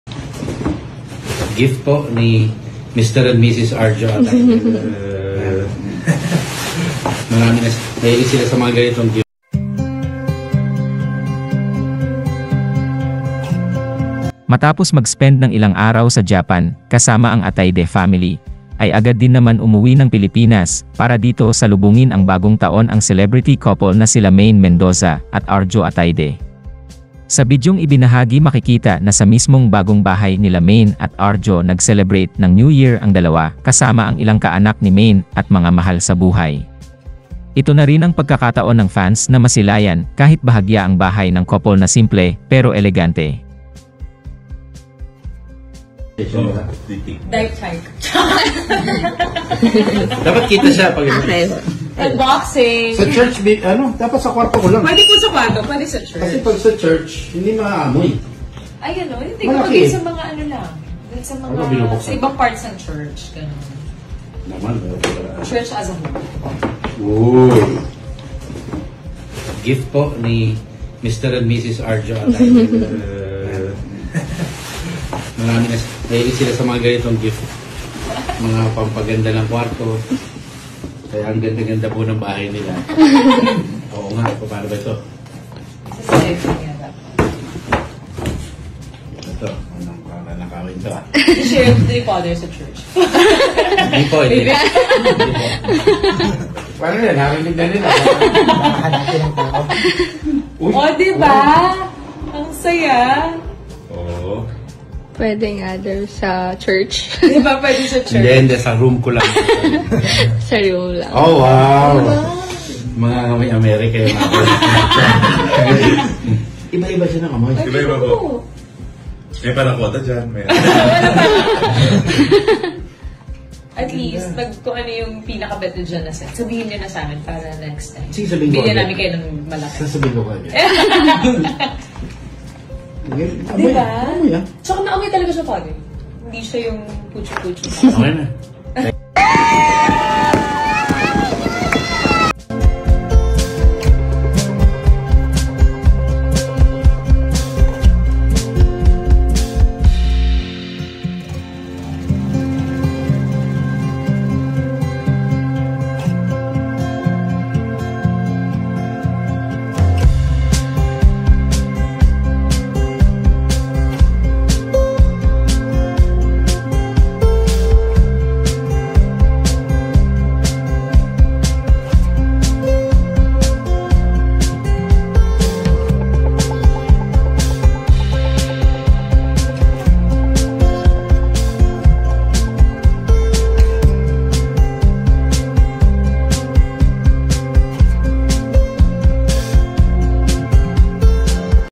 MAINE & ARJO NEW HOUSE 2024 NEW YEARS EVE CELEBRATION WITH THE MENDOZA FAMILY
arjo atayde and maine mendoza new house 2024
maine mendoza house tour